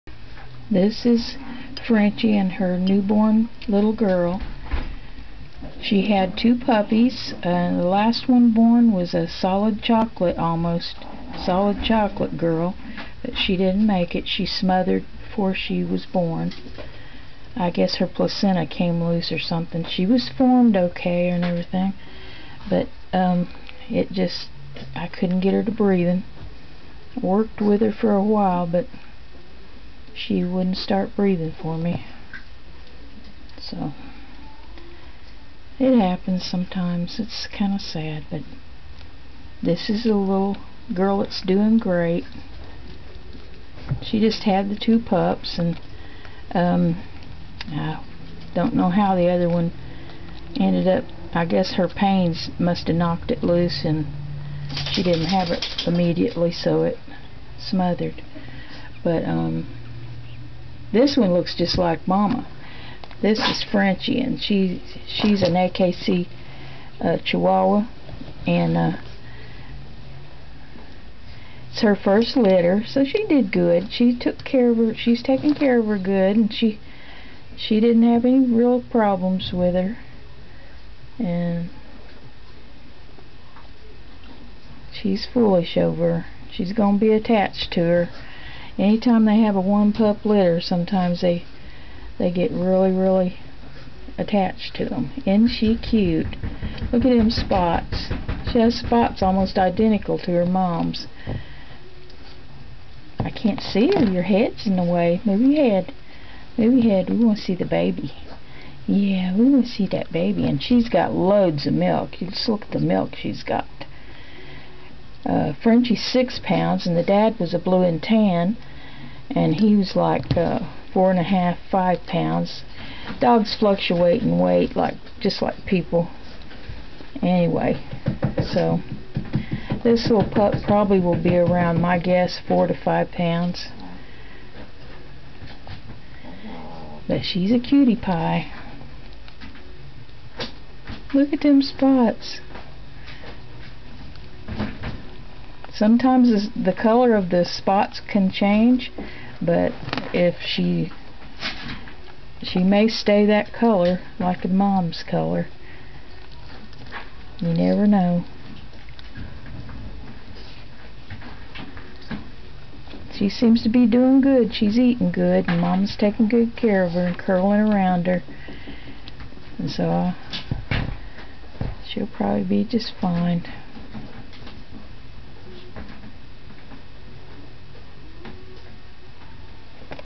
LKLs October Gold Chihuahuas, AKC & CKC Frenchy's New Girl Oct13 2019
My AKC Applehead Chihuahua had 2 girls one is alive.
She is just like her mom... So Cute!
Pick Up is here. Chatt.,Tenn. area 37341 ( Harrison, Tenn.)